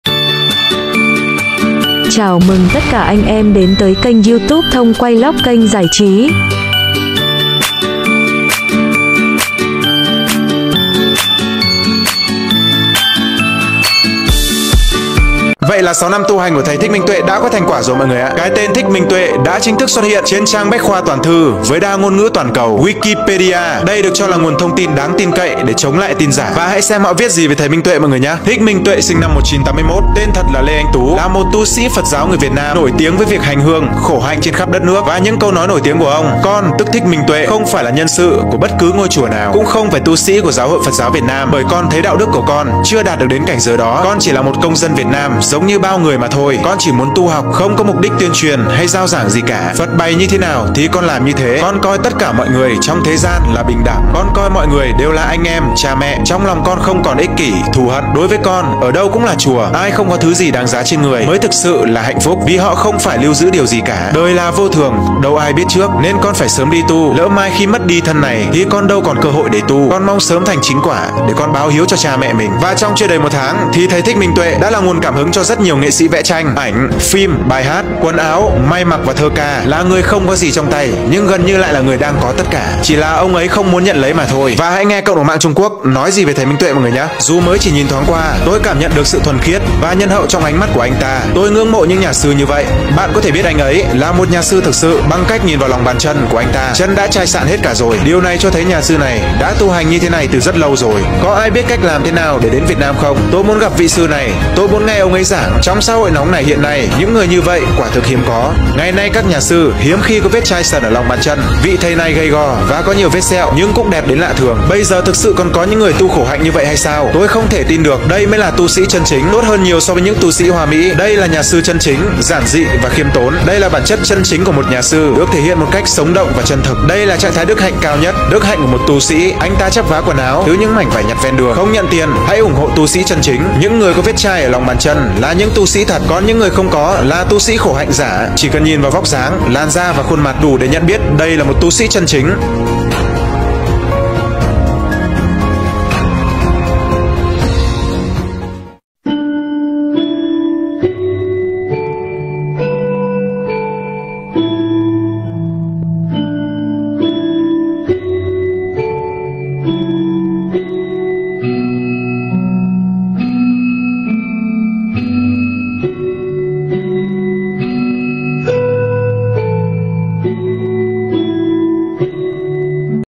31/5 trời đây mưa gió ( các sư thây đi trong mưa )
Hàng ngàn người dân đứng hai bên đường ko ngại mưa gió và mấy anh chiến sĩ hộ tống và điều tiết giao thông
Chân thành cám ơn rất nhiều bà con và tất các mấy anh chiến sĩ